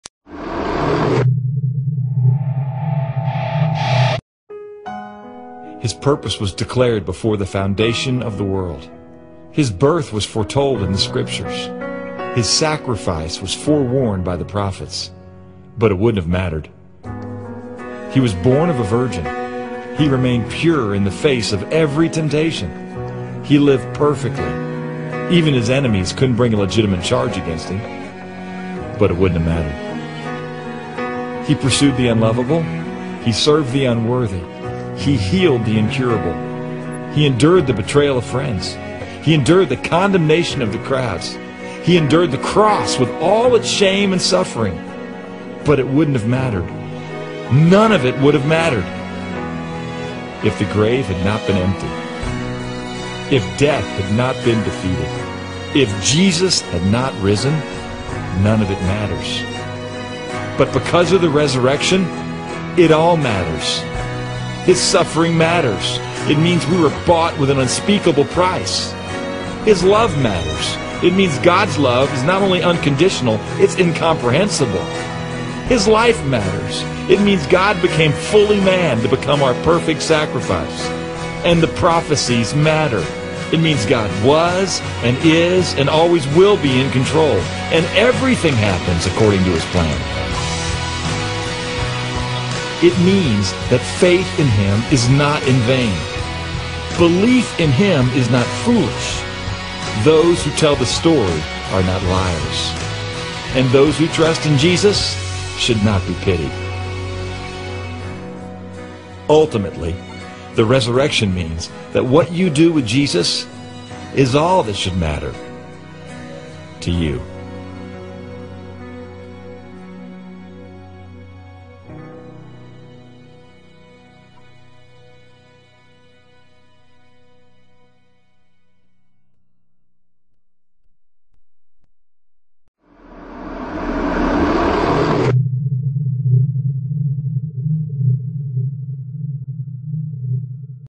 It Wouldn't Have Mattered
"Death is swallowed up in victory. O death, where is your victory? O death, where is your sting?" For sin is the sting that results in death, and the law gives sin its power. How we thank God, who gives us victory over sin and death through Jesus Christ our Lord! So, my dear brothers and sisters, be strong and steady, always enthusiastic about the Lord's work, for you know that nothing you do for the Lord is ever useless.'
1 Corinthians 15:55-58